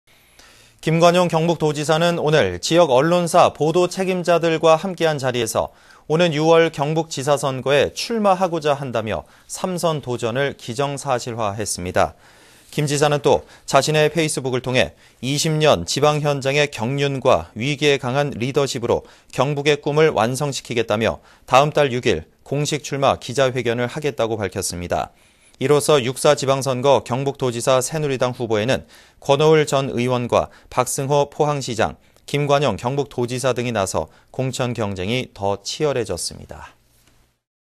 [안동MBC뉴스]김관용 경북도지사, 지방선거 출마 공식 선언
2014/02/26 17:48:11    작성자 : 보도팀장

 김관용 경북도지사는 오늘
지역 언론사 보도책임자들과 함께한 자리에서
오는 6월 경북 지사 선거에 출마하고자 한다며
3선 도전을 기정사실화했습니다.
           ◀ＥＮＤ▶

김 지사는 또 자신의 페이스북을 통해
20년 지방현장의 경륜과
위기에 강한 리더십으로
경북의 꿈을 완성시키겠다며
다음 달 6일 공식출마 기자회견을 하겠다고
밝혔습니다.

이로써 6·4 지방선거 경북도지사
새누리당 후보에는
권오을 전 의원과 박승호 포항시장,
김관용 경북도지사 등이 나서 공천 경쟁이
더 치열해졌습니다.